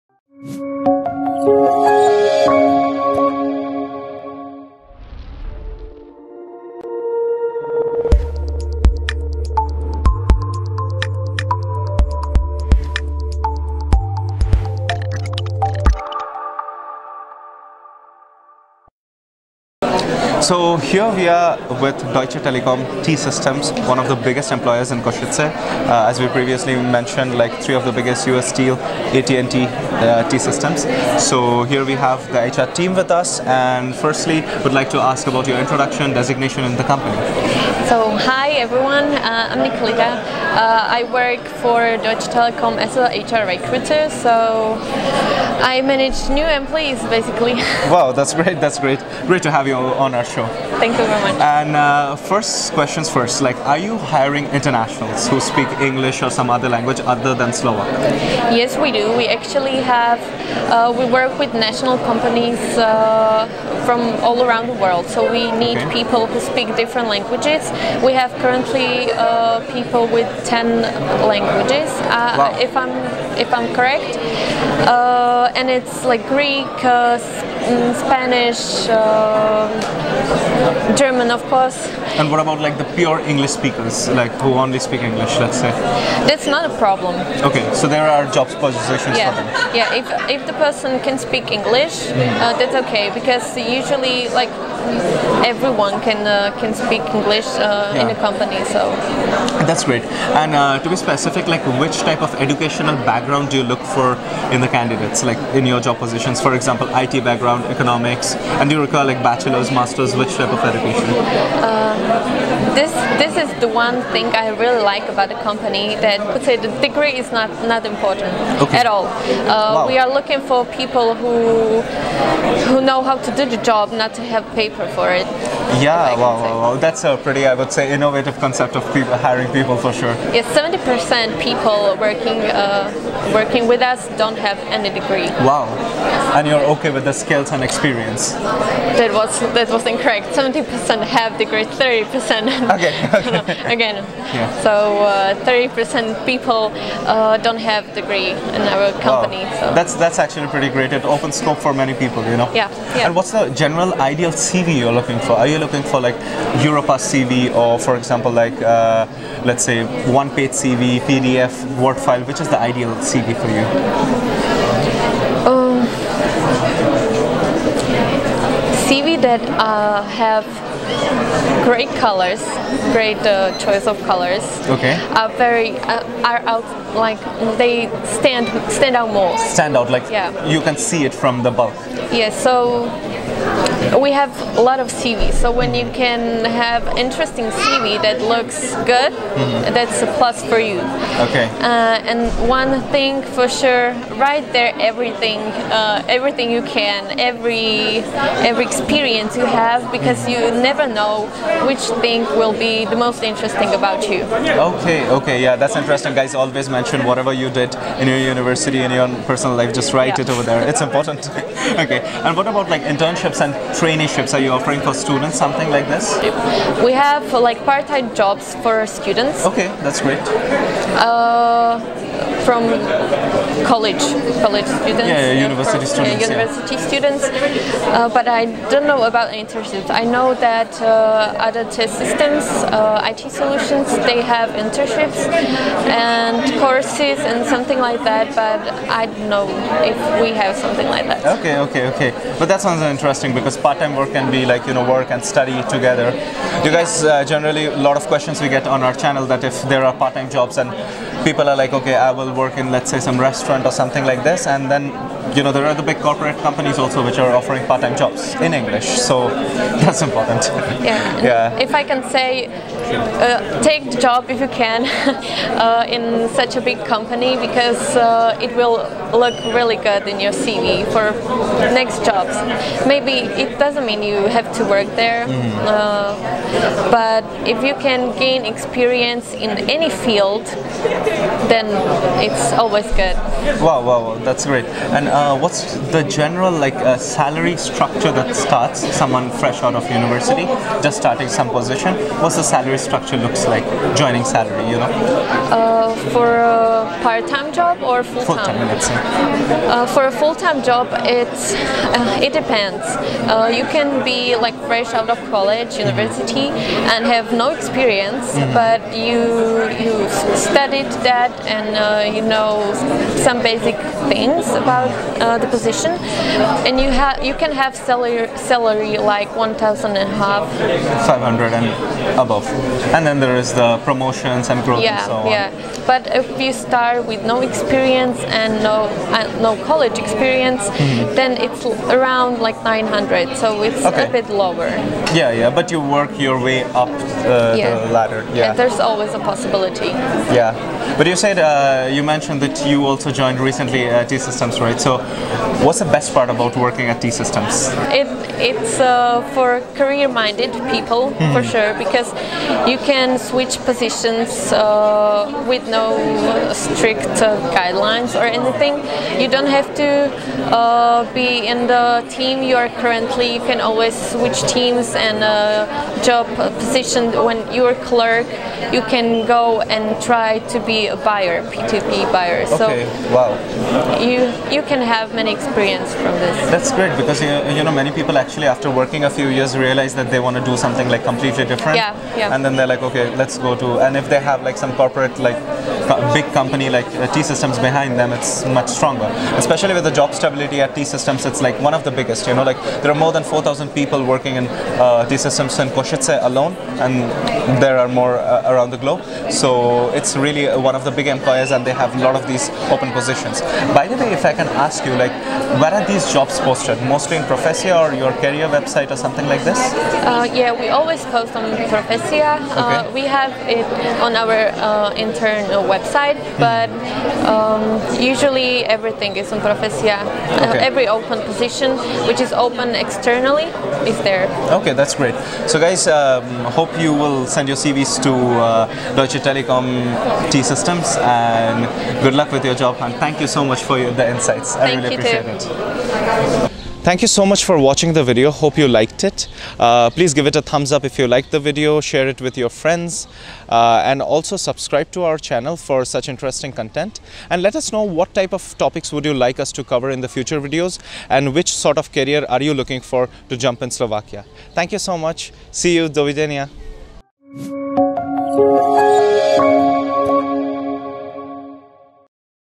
Foreigner-friendly jobs: T-Systems Slovakia (Deutsche Telekom)
One of the biggest employers in Slovakia has over 3900 employees now. The history of the company started in 2007 in Kosice. It is a company with one of the highest growth in Slovakia constantly looking for talented and educated people from all over the world. There are both Junior and Senior positions offered in Slovakia.